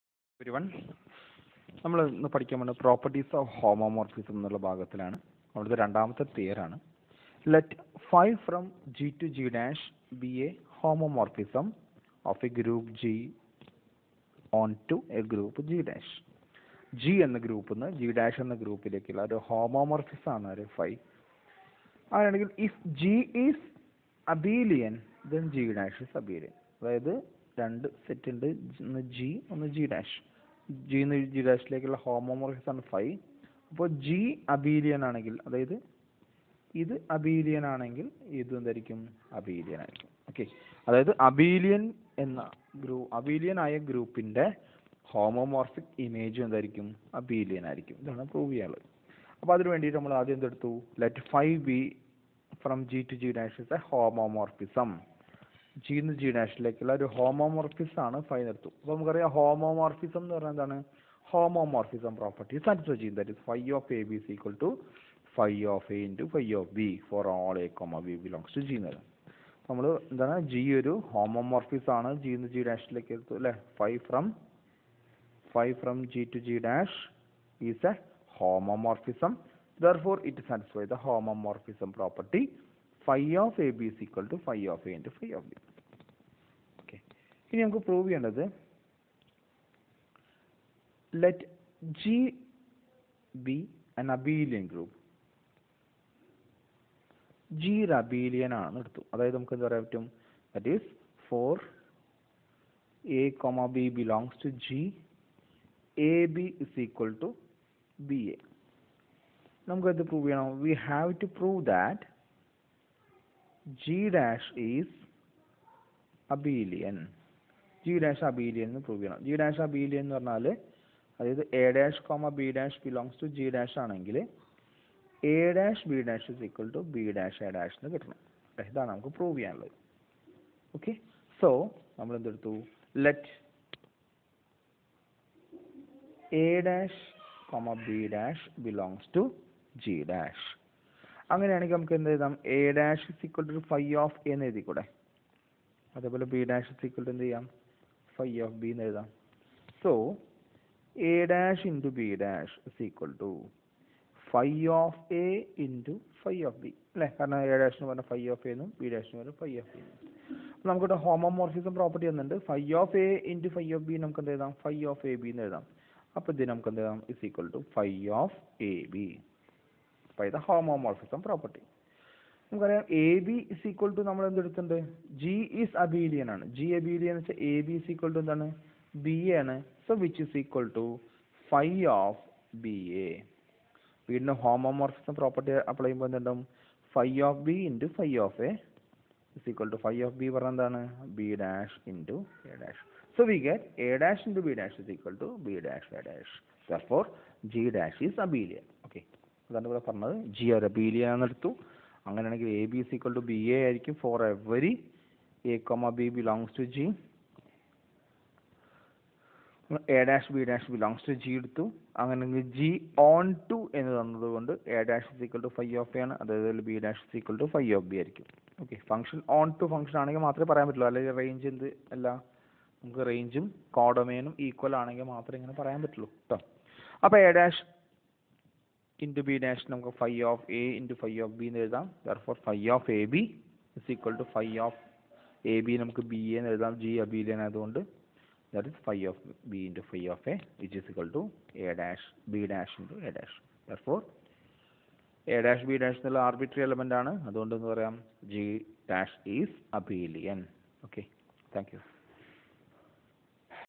Theorem: if Phi is a homomorphism from G ONTO G'.if G is abelian then G' is also abelian
Group theory